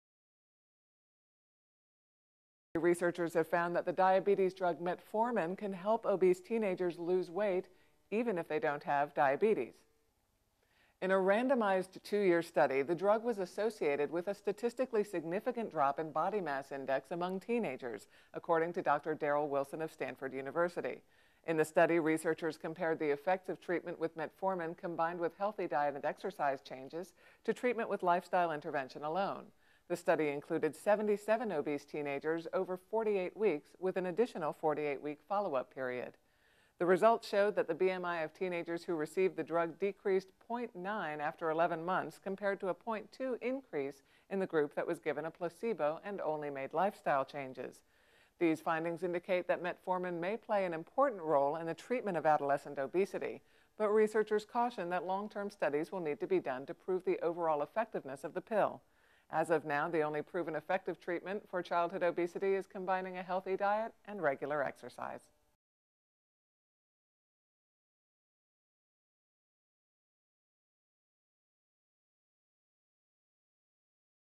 Metformin May Help Obese Teens Lose Weight
Researchers have found that the diabetes drug metformin can help obese teenagers lose weight, even if they dont have diabetes.